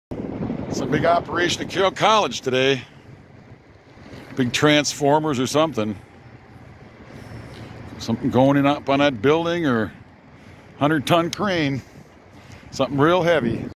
Today behind Carroll Uni.. 100 ton crane!
On a bike ride today and saw this behind Carroll University!

Please leave any ideas, suggestions or recommendations in the comments below!